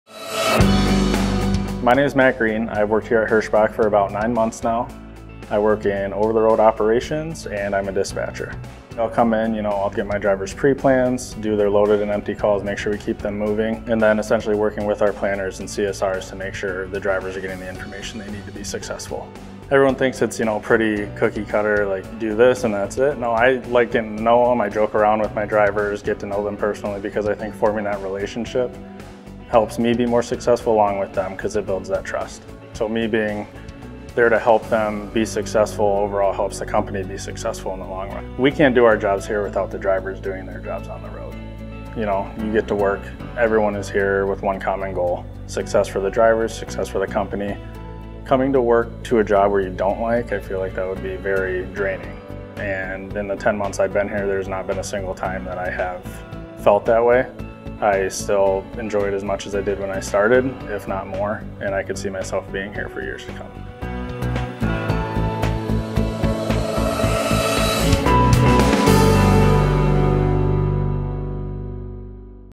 Hirschbach: Who We Are - Matt Greene
Meet Matt Greene, OTR Driver Manager and Dispatcher since 2021, as we learn about the importance of his connection with our drivers. Are you ready to be All in to Win? Apply Now . Get your career rolling with Hirschbach!